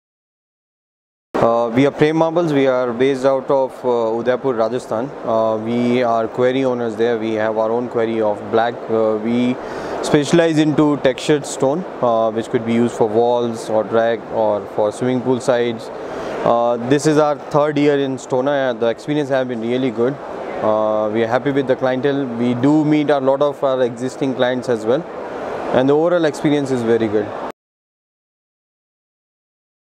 Mr. Dev Sing is pleased to meet existing and new clientele during their 3rd edition in STONA.
Mr. Dev Singh (Exhibitor) is pleased to meet existing and new clientele during their 3rd edition in STONA.